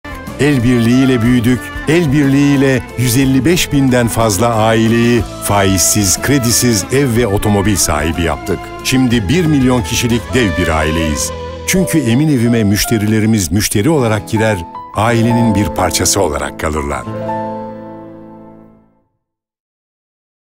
1991’den beri Elbirliği’yle…
29 yıl önce, Elbirliği Sistemi’ni icat ederken biz, geleneklerimizden ilham aldık. Ne yaptıysak ülkemiz için yaptık. Elbirliği’yle büyüdük, Elbirliği’yle 155.000'den fazla aileyi faizsiz, kredisiz ev ve otomobil sahibi yaptık. Şimdi 1 milyon kişilik dev bir aileyiz.